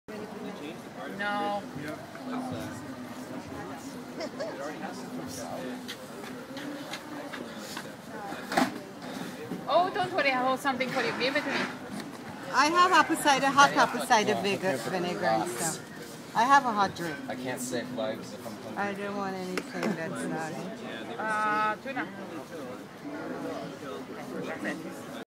Raw Video: Occupy Toronto in St. James Park PT6 - November 13 2011
Raw Video DBE Media Group - Occupy Toronto in St. James Park PT6 - November 13 2011 Stock Video

Recorded By David Epstein on his iPhone 4